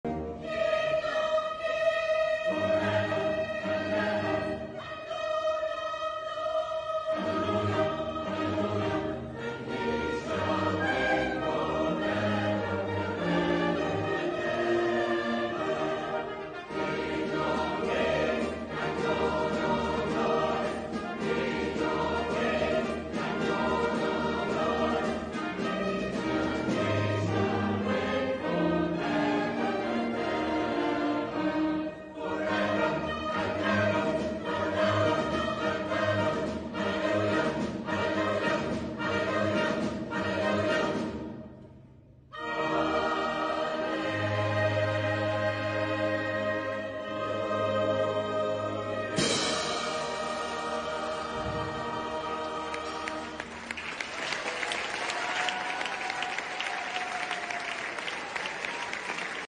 An evening of glorious music
They are back! The Manama Singers and the newly-formed Bahrain Chamber Orchestra performed a sell out concert at The American School of Bahrain.

The famous Manama Singers Wall of Sound was just as glorious as ever when it came into play and the more delicate renditions of well-known favourites had us all pinned entranced in our seats.